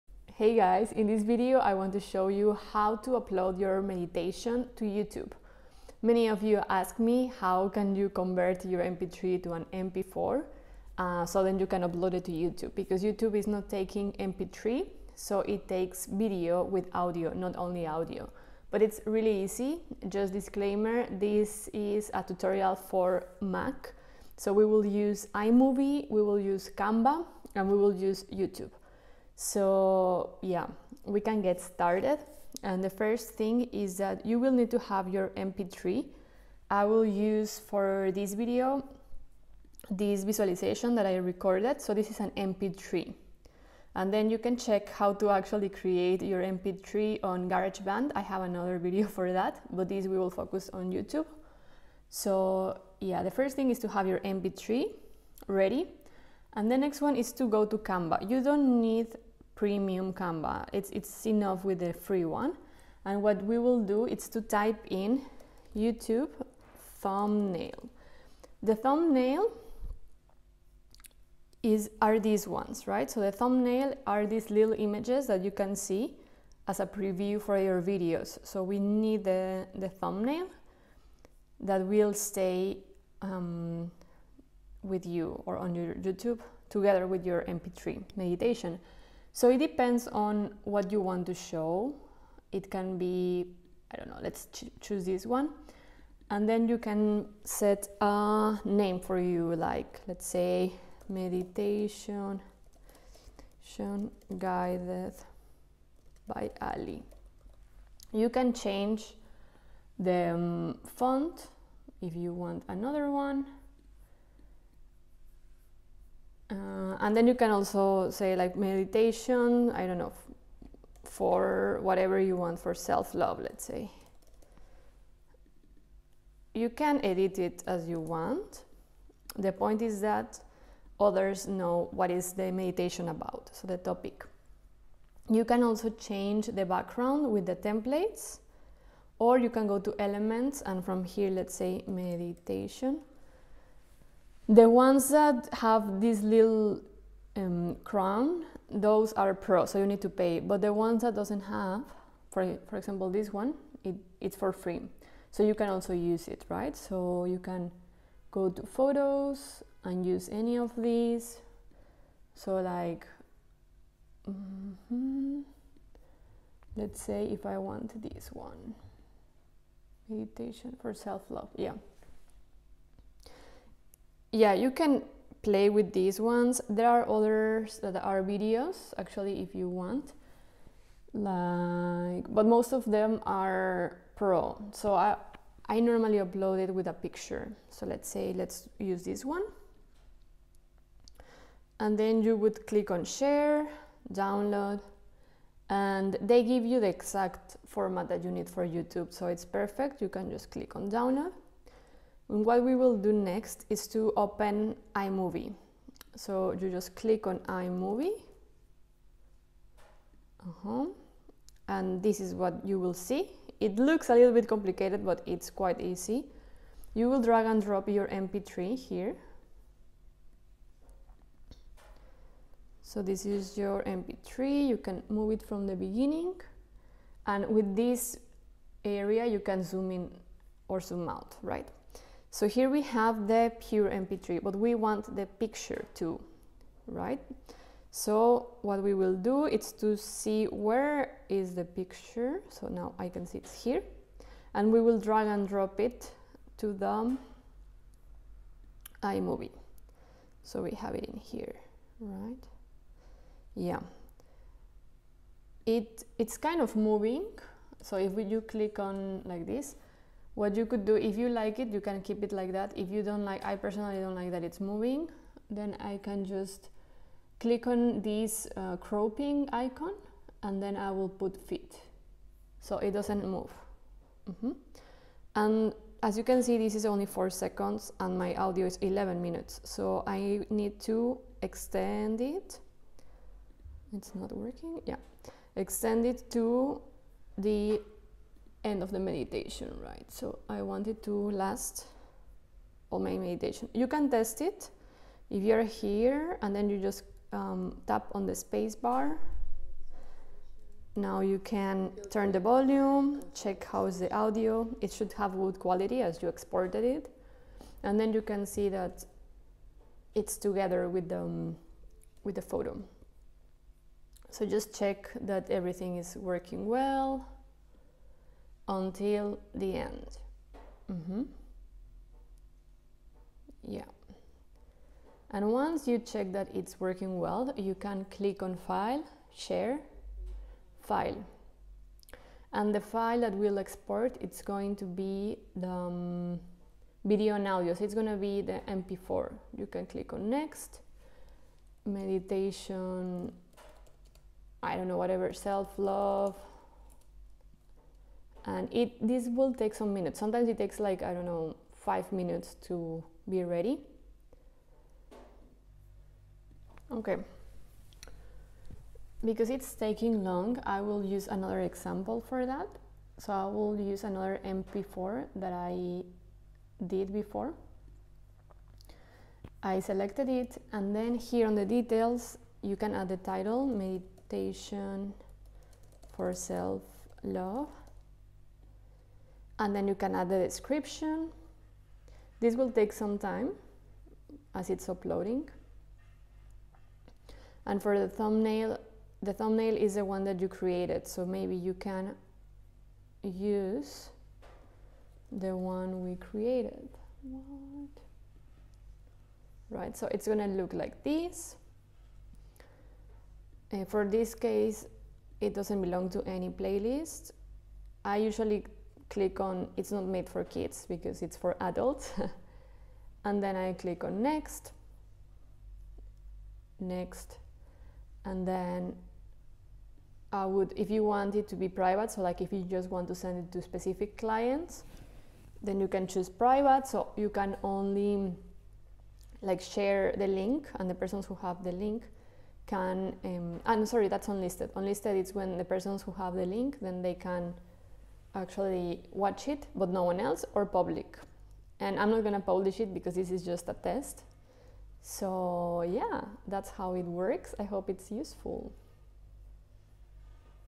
Create video meditations for YouTube (iMovie tutorial)
YouTube is a great way to share your meditations, and learn how to create an audio meditation to a video one with iMovie (for Mac):

00:00 intro
01:18 Canva tutorial
03:26 iMovie tutorial
05:56 Upload to YouTube